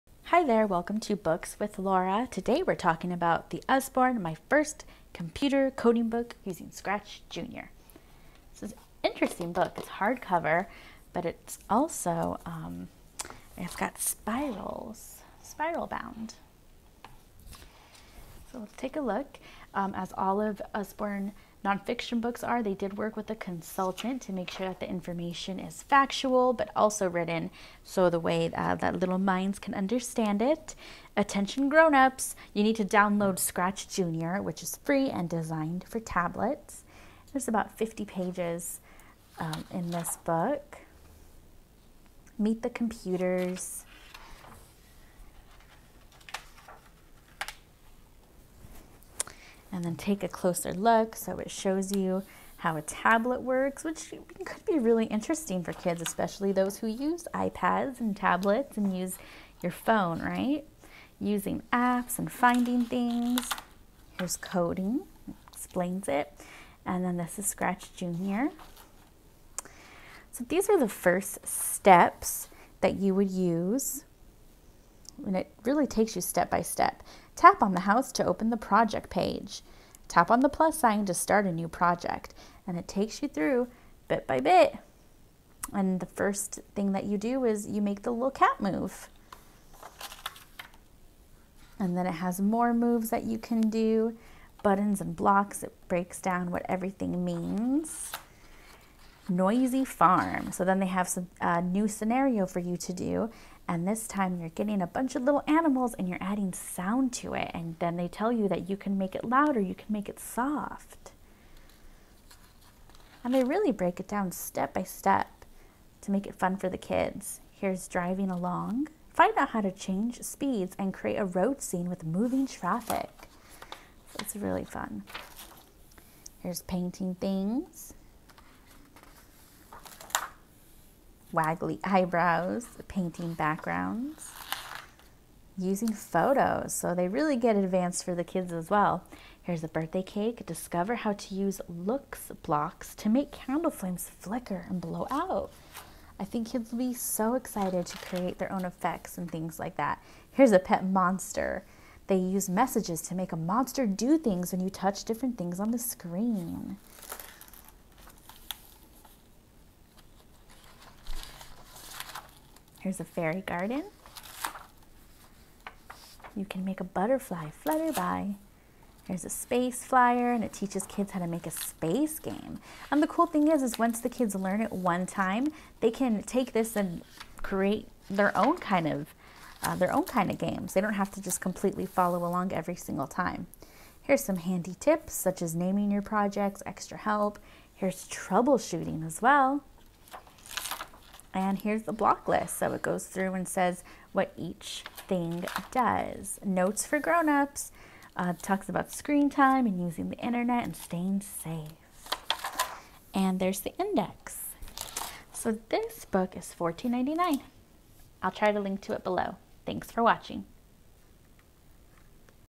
My First Computer Coding Book Using Scratch Jr. 📱 Usborne Books & More
If your child can't get enough of a tablet/phone, why not use their interest and plant a seed for learning? This invaluable resource will help teach your child how to code using Scratch Jr. a free coding app for beginners.

"This fun, friendly guide is a perfect first introduction to computer coding. Discover how computers work and what coding does – then follow simple, step-by-step instructions to code your own stories and games. The coding uses Scratch Jr., a computer language designed for beginners and available for free on computers and tablets. Helpful notes for grown-ups included!" - Amazon

Find this book here on Amazon or from an Usborne Books & More consultant